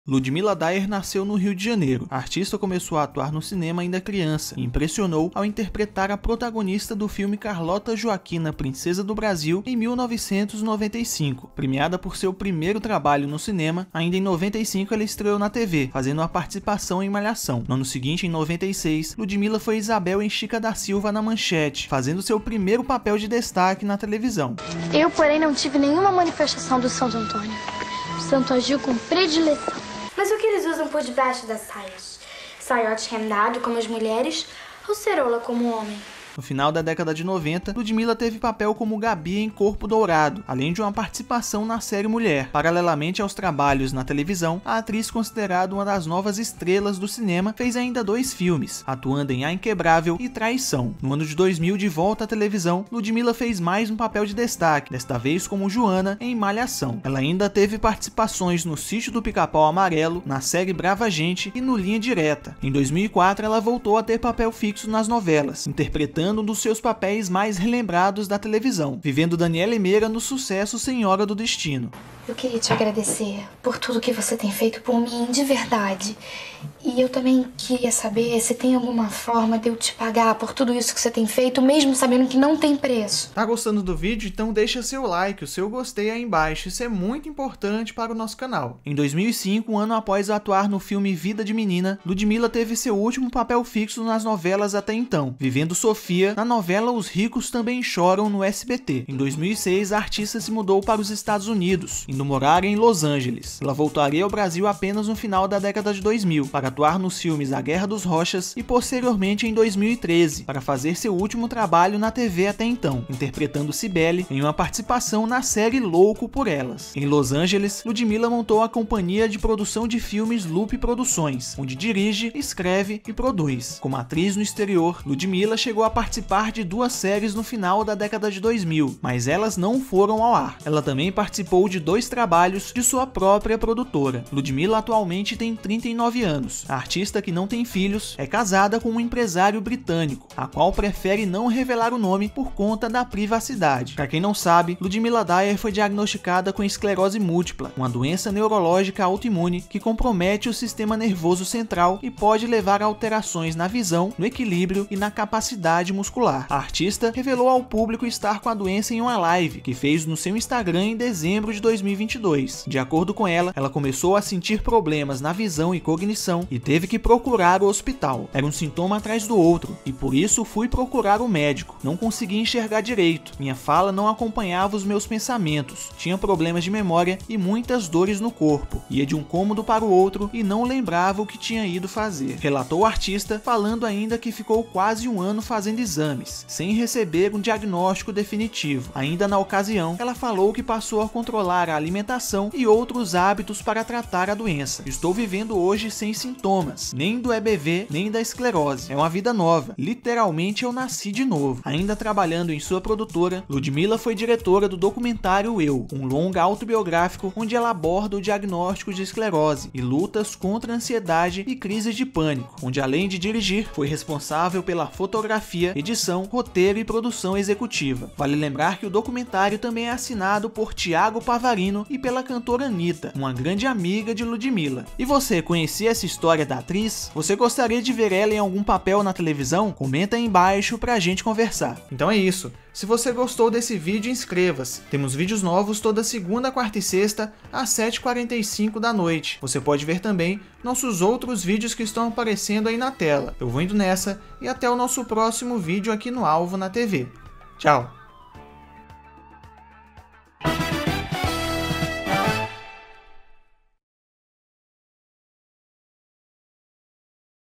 COMO ESTÁ LUDMILA DAYER, A DANIELLE DE SENHORA DO DESTINO
Neste vídeo você vai ver por onde anda a atriz Ludmila Dayer, a Danniele de Senhora do Destino.

Ludmila Dayer nasceu no Rio de Janeiro capital.
A artista, começou a atuar no cinema ainda criança e impressionou ao interpretar a protagonista do filme "Carlota Joaquina, Princesa do Brasil", de 1995. Premiada por seu primeiro trabalho no cinema, ainda em 1995 ela estreou an televisão, fazendo participação em "Malhação". No ano seguinte, em 1996, Ludmyla foi Isabel em "Xica da Silva" da rede Manchete, fazendo seu primeiro papel de destaque na televisão.

No final da década de 1990, Ludmilla teve papel como Bibi em "Corpo Dourado", além de participação na série "Mulher". Paralelamente aos trabalhos na televisão, a atriz considerada uma das novas estrelas do cinema, fez ainda dois filmes, atuando em "A Inquebrável" e "Traição", este último, bastante premiado.
No ano de 2000, de volta a televisão, Ludmila teve outro papel de destaque, desta vez como Joana Vasconcellos em "A Malhaççao".
Em anos anos posteriores a "Malhação", a artista teve participações nas produções como "Sítio do Pica Pau Amarelo", "Brava Gente" e "Linha Direta", até que em 2004, Ludmyla voltou a ter papel fixo nas novelas, interpretando um dos seus papéis mais relelmbrados da televisão, vivendo Danielle Meira no sucesso "Senhora do Destino".